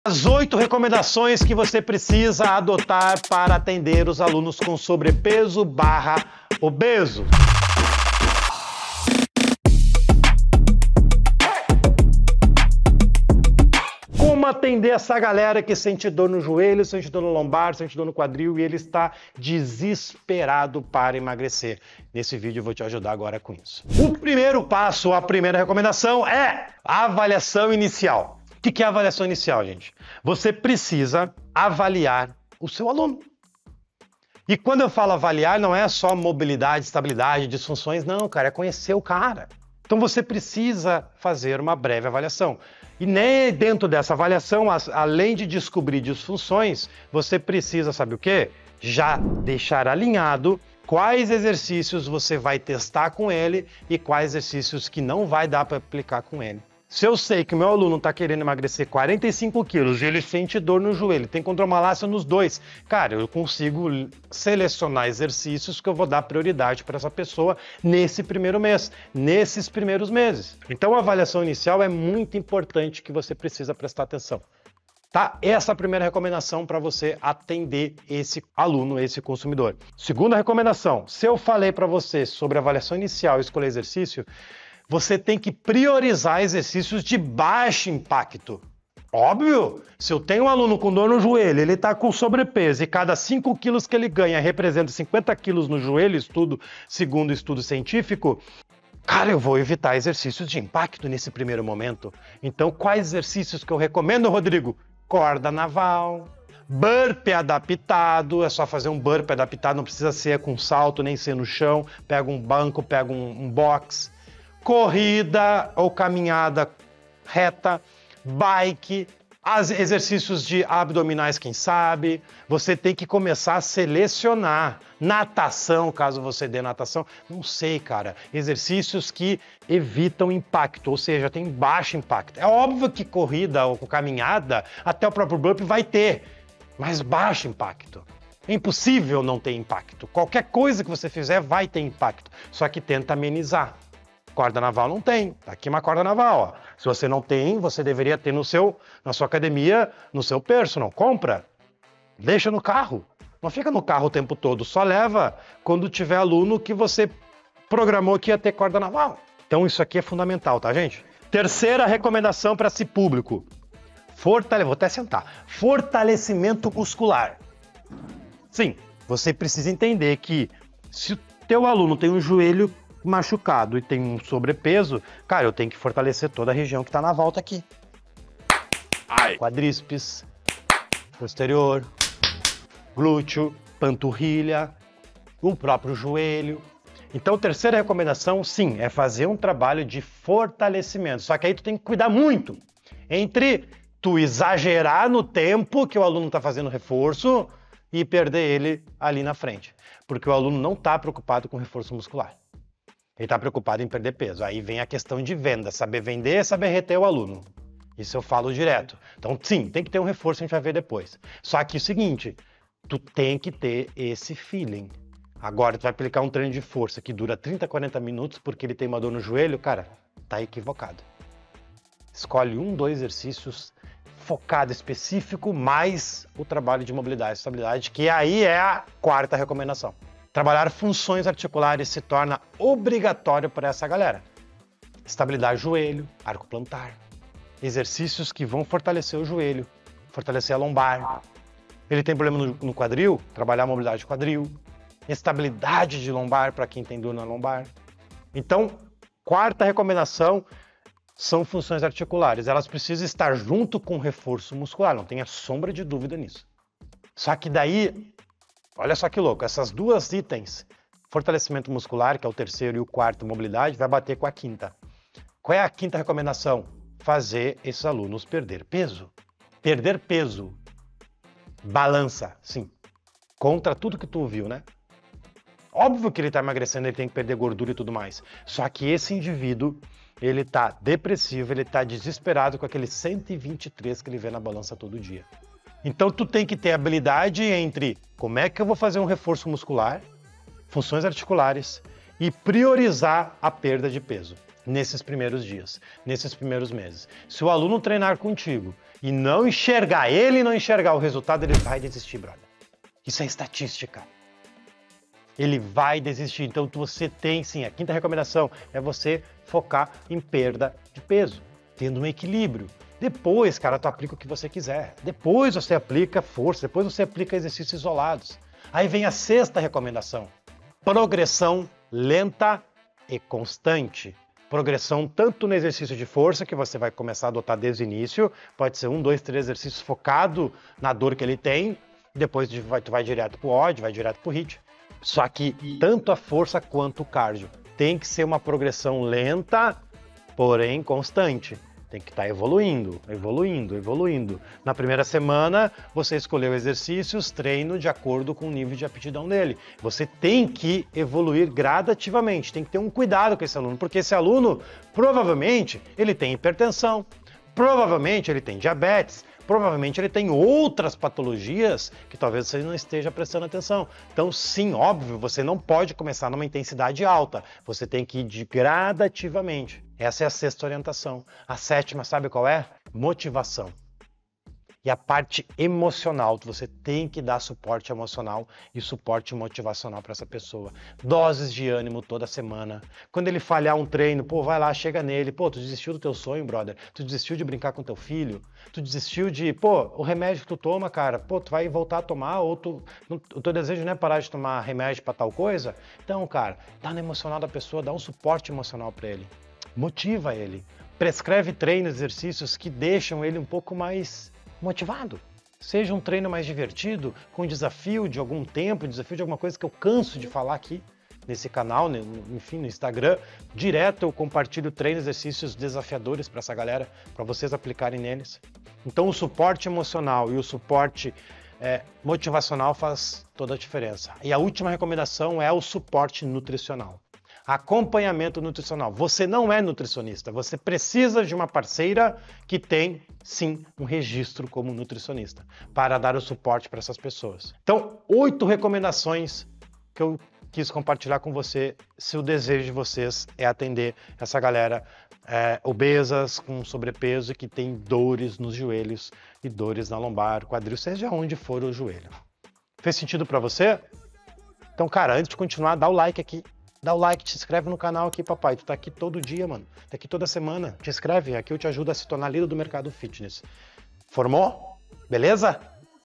COMO TREINAR O SEU ALUNO COM SOBREPESO!
8 RECOMENDAÇÕES PARA APLICAR EM ALUNOS COM SOBREPESO!
COMO AJUDAR SEU ALUNO COM OBESIDADE!
8 passos para treinar o seu aluno com obesidade / sobrepeso

Como atender essa galera?
Rodrigo meu aluno tem dor na lombar, meu aluno tem dor no quadril? meu aluno tem sobrepeso e sente dores nos joelhos?

Calma papai! Vamos lá...
Vou te sugerir 8 recomendações para você entender, orientar e entregar o resultado que o seu aluno precisa, bora!

1º: Avaliação inicial
2º: Exercícios de baixo impacto
3º: Fortalecimento muscular
4º: Trabalhe funções articulares
5º: Faça ele perder peso
6º: Progressão lenta e constante
7º: Motivação do aluno
8º: Suporte nutricional

Avaliação inicial: Deixe alinhado com o seu aluno quais são os exercícios que podem ser feitos e quais não podem!

Exercícios de baixo impacto: Corda naval, burpee adaptado, corrida ou caminhada. Tente AMENZAR o impacto.

Fortalecimento muscular: Se o teu aluno tem dores nos joelhos, dores no quadril, porque não fortalecer primeiro a região onde ele está com maiores dores?! Só não exagere com o tempo dos treinos... isso precisa de um "feeling" teu!

Trabalhe funções articulares: Faça isso junto com os reforços musculares!

Faça ele perder peso: Agora complicou... é! Tu tem que ter a habilidade de trabalhar funções articulares, trabalhar força onde precisa e ao mesmo tempo tem que estar fazendo o teu aluno perder peso. Porque ele PRECISA ver isto na balança e no espelho.

Progressão lenta e constante: Tanto no exercícios de força quanto o cardio, isso precisa ser evolutivo! E tem que cuidar com as doenças que ele já tem, muito importante!

Motivação do aluno: Não tem como não falar sobre isso né gente, você precisa ajudar e lembrar ele(a) sobre o porque ele(a) está fazendo aquilo com você!

Suporte nutricional: Alimentação é CRUCIAL para ele ver a diferença fora das aulas com você então, procure uma parceira(o) nutri e / ou indique para o seu aluno!